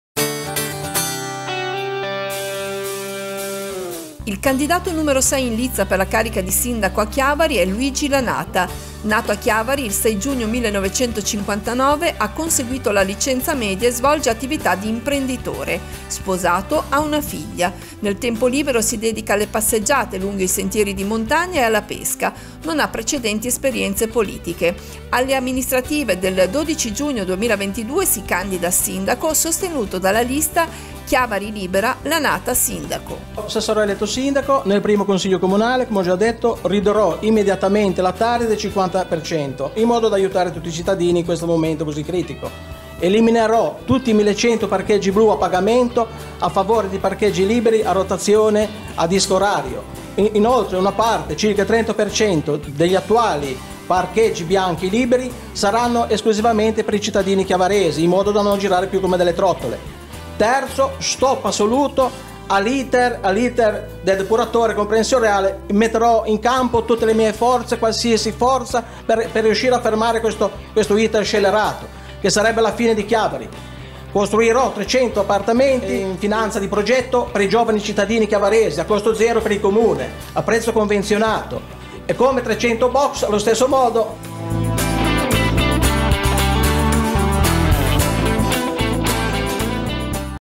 Elezioni 2022, Chiavari: il candidato sindaco Luigi Lanata
Luigi Lanata, imprenditore, alla prima esperienza politica, scende in campo nella competizione elettorale a Chiavari. I punti del programma: tari, parcheggi, depuratore, case per i giovani.